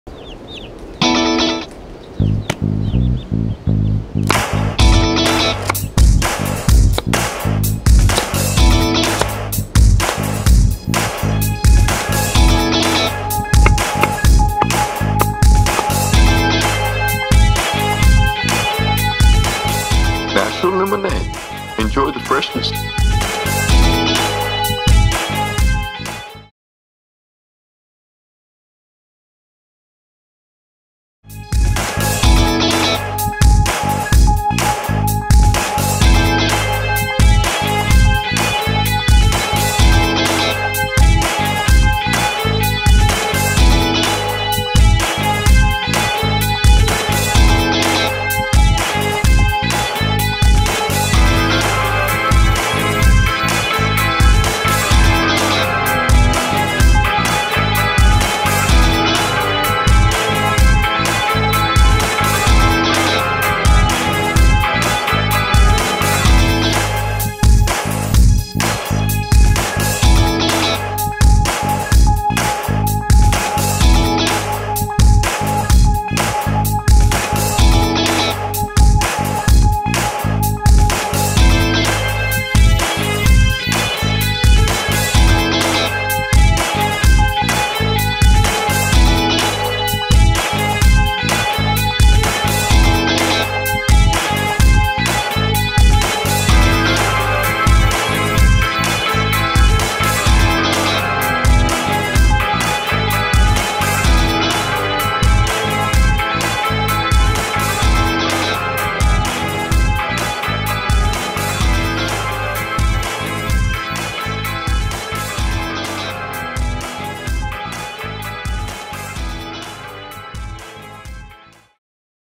Natural Lemonade Non commercial advertisement
A non commercial advertisement from Dream vision productions.
Cast: Hamed Shahkarami
Camera: Shahab Daie
3D Effects and compositing: Hamid Ghanbari and Saied Rahmani
Edit: Saied Rahmani
Music and Sound Effects: Na-G